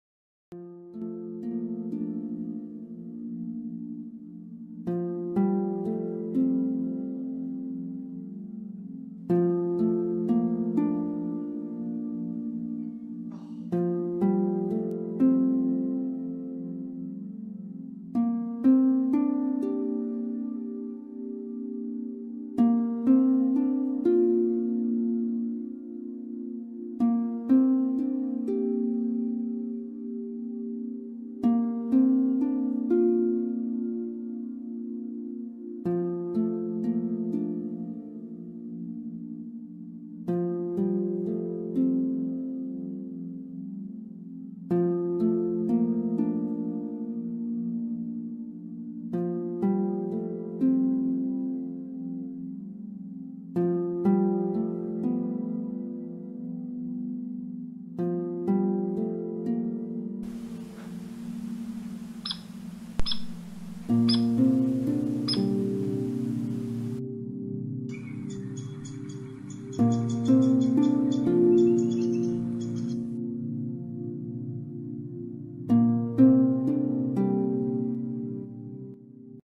4 Différences entre le Pic chevelu et le Pic mineur (Québec)🐦
Les vidéos ont été prises à Chicoutimi (Québec).